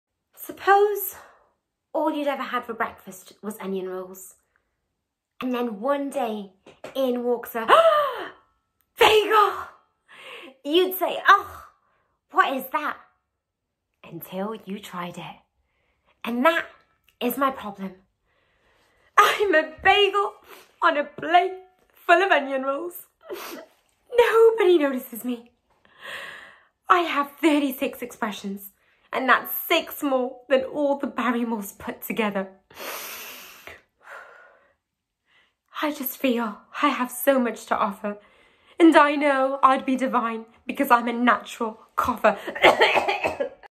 I’m a bagel monologue
By Isobel Lennart
Funny Girl Monologue
**Has been condensed, not the original full version**